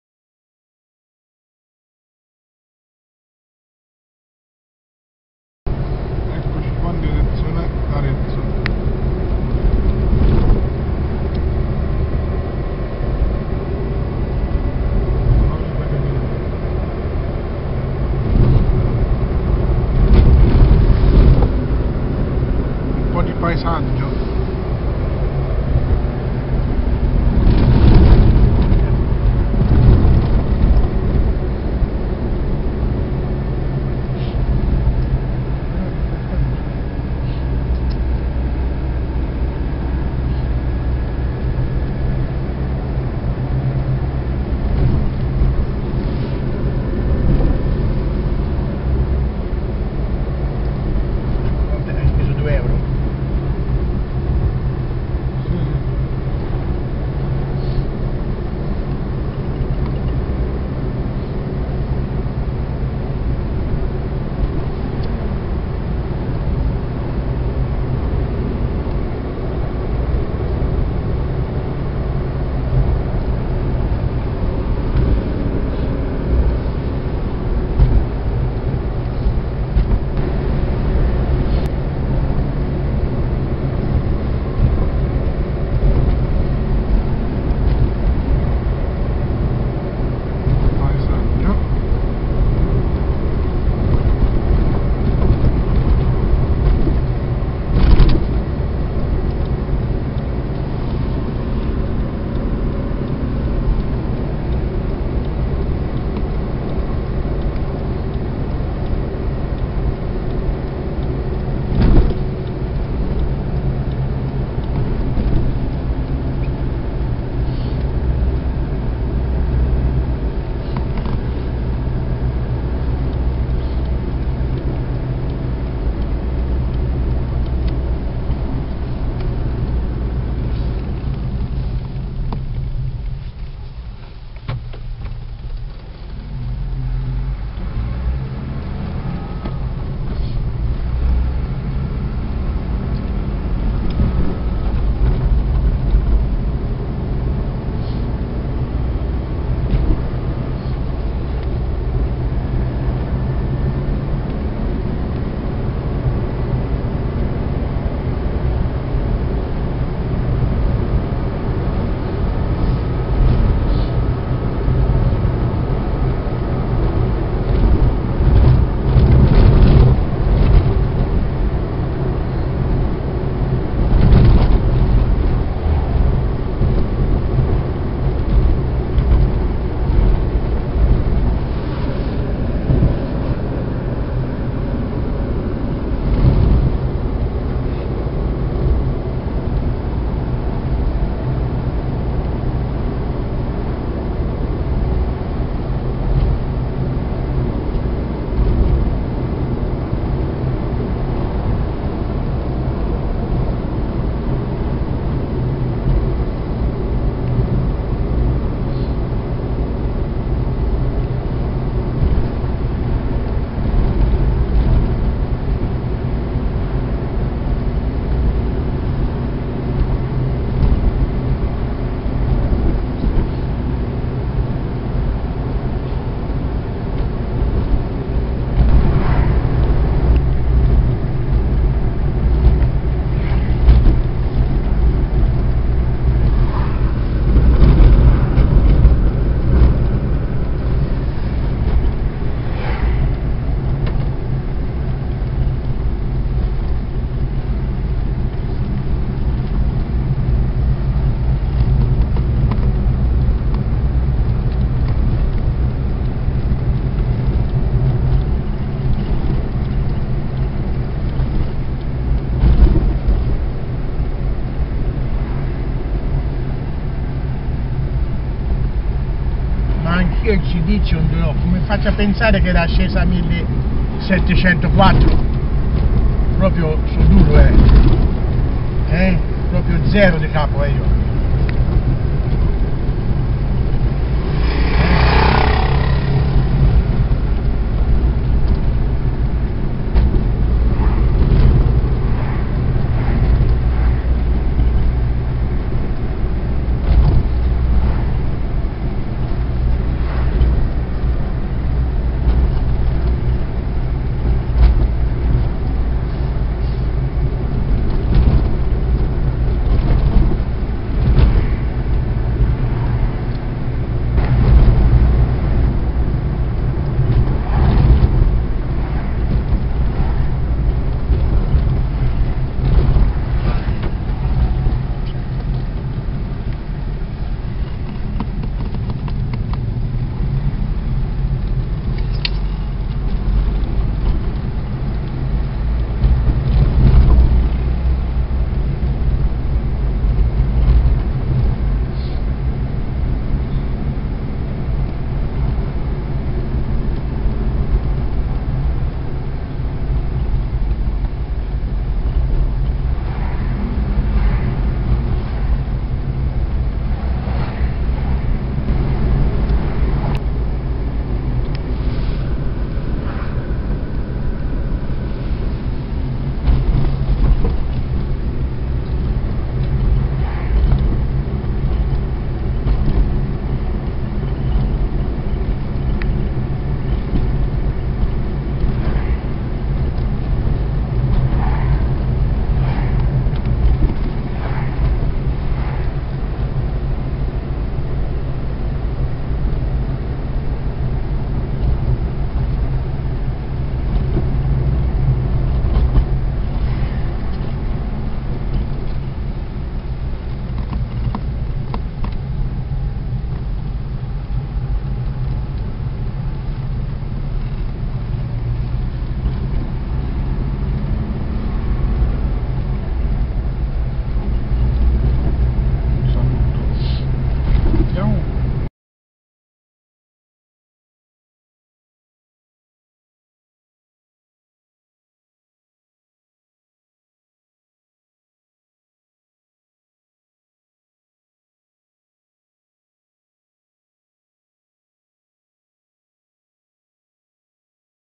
Filmato - In Auto In Direzione verso Arezzo
Un salutone Amici di Tele Babu Web Tv , Questo è il video girato in Auto in Direzione da Rigutino (AR) Verso Arezzo ! Spero sia di Vostro gradimento ! Un salutone e a presto con nuovi Video !